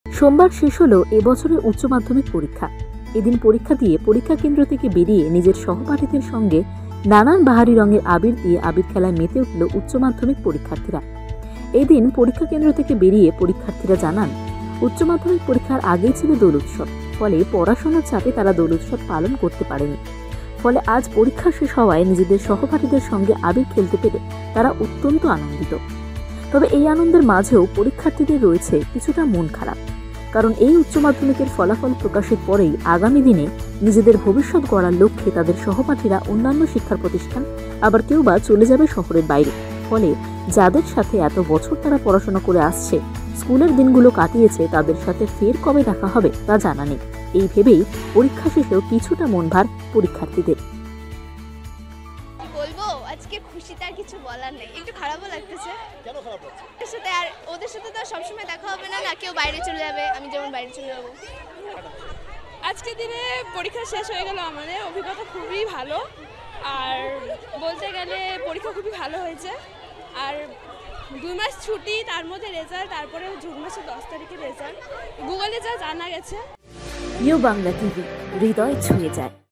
উচ্চমাধ্যমিক পরীক্ষা শেষেও মনভার পরীক্ষার্থীদের | Siliguri | U Bangla TV
*উচ্চমাধ্যমিক পরীক্ষা শেষেও মনভার পরীক্ষার্থীদের*

*শিলিগুড়ি, পার্থ প্রতিম দাস, ২৭শে মার্চ:* সোমবার শেষ হল এবছরের উচ্চমাধ্যমিক পরীক্ষা। এদিন পরীক্ষা দিয়ে পরীক্ষাকেন্দ্র থেকে বেরিয়ে নিজের সহপাঠিদের সঙ্গে নানান বাহারি রঙের আবির দিয়ে আবির খেলায় মেতে ওঠল উচ্চমাধ্যমিক পরীক্ষার্থীরা। এদিন পরীক্ষাকেন্দ্র থেকে বেরিয়ে পরীক্ষার্থীরা জানান, উচ্চমাধ্যমিক পরীক্ষার আগেই ছিল দোল উৎসব ফলে পড়াশোনার চাপে তারা দোল উৎসব পালন করতে পারেনি, ফলে আজ পরীক্ষা শেষ হওয়ায় নিজেদের সহপাঠীদের সঙ্গে আবির খেলতে পেরে তারা অত্যন্ত আনন্দিত। তবে এই আনন্দের মাঝেও পরীক্ষার্থীদের রয়েছে কিছুটা মন খারাপ। কারণ এই উচ্চমাধ্যমিকের ফলাফল প্রকাশের পরেই আগামী দিনে নিজেদের ভবিৎসত গড়ার লক্ষে তাদের সহপাঠিরা অন্যান্য শিক্ষাপ্রতিষ্ঠান আবার কেউবা চলে যাবে শহরের বাইরে, ফলে যাদের সাথে এত বছর তারা পড়াশোনা করে আসছে স্কুলের দিন গুলো কাটিয়েছে তাদের সাথে ফের কবে দেখা হবে তা জানা নেই, এই ভেবেই পরীক্ষা শেষেও কিছুটা মণভার পরীক্ষার্থীদের।@ubanglatvofficial